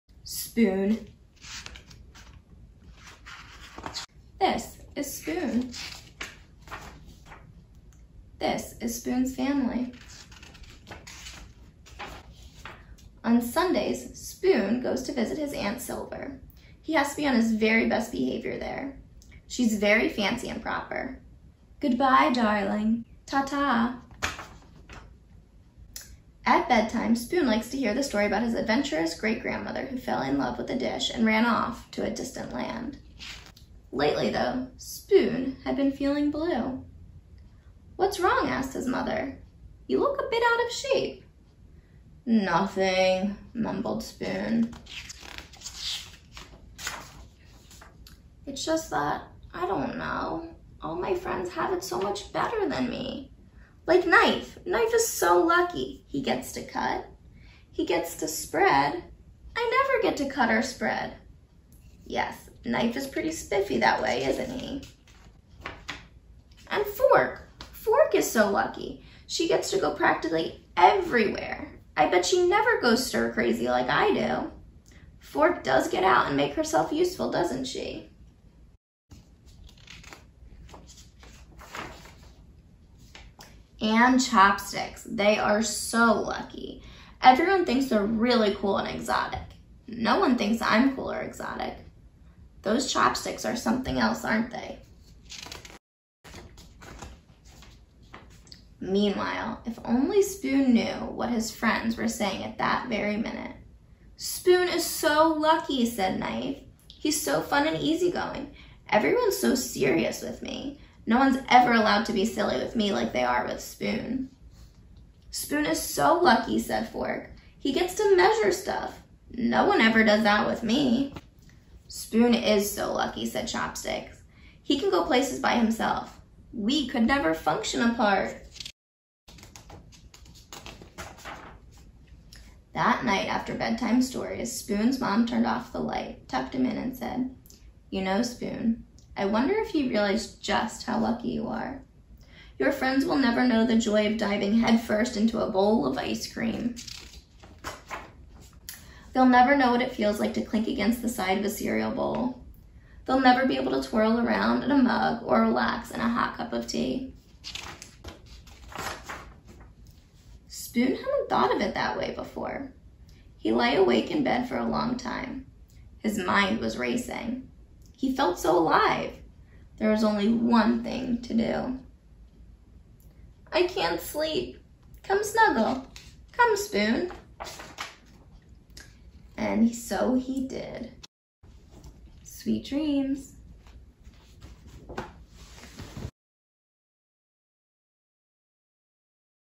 Spoon by Amy Krouse Rosenthal (Read by Laura
A punny goodnight story that celebrates uniqueness